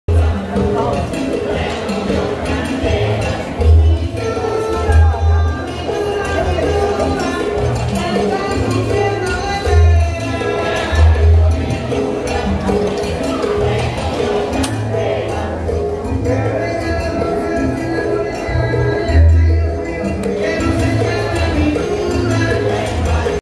Inside Marché Bon Secours window view at the Montreal Salsa Convention on May 18th 2014
Inside Marché Bon Secours window view of the old port of Montreal at the Montreal Salsa Convention on May 18th 2014

Montreal, Canada